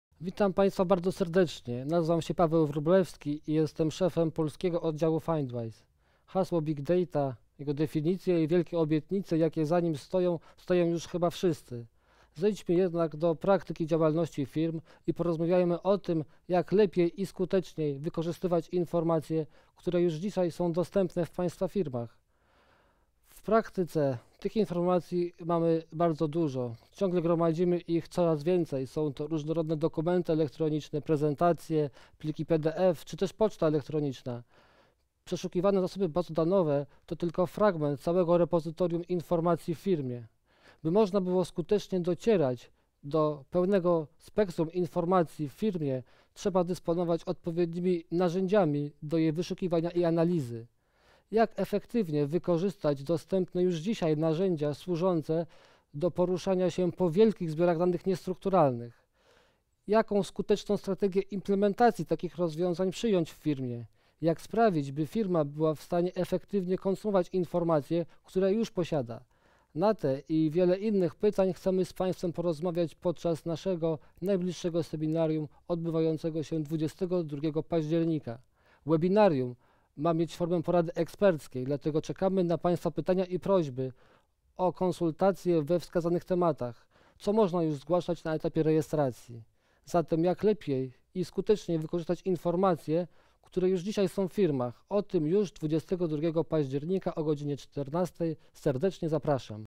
BigData – jak wyszukać i analizować dane niestrukturalne?
Zapoznaj się z BigData w kontekście wyszukiwania i analizy informacji niestrukturalnych i weź udział w webinarium. Zaproszeni eksperci firmy Findwise czekają, by podjąć temat. Firma Findwise specjalizuje się w usługach doradczych związanych z wdrażaniem różnorodnych rozwiązań wyszukiwania informacji w przedsiębiorstwie. Wejdź na stronę by zobaczyć najbliższe webinaria.